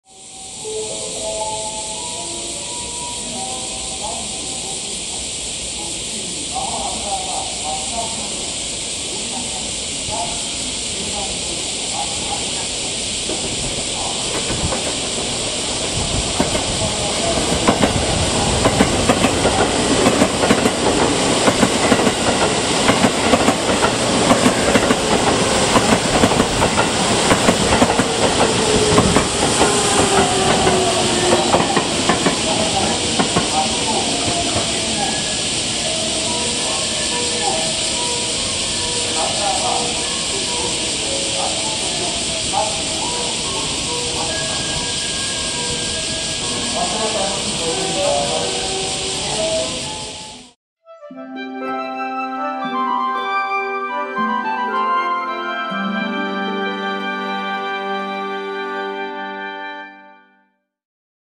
JR京都線223系快速網干行き 新大阪到着！
JR京都線（東海道本線）新大阪駅8番のりばに到着する快速網干行。223系8両編成。
土休日ダイヤではすべての大阪方面への快速は8番のりばに到着。元々は分岐器直線側の本線であったが、おおさか東線用のホームを増設した際に東に移動し、普通が停車する7番のりばが本線となった。このため、快速と、外側線から乗り入れる新快速は当駅停車前と発車後に速度制限が入る。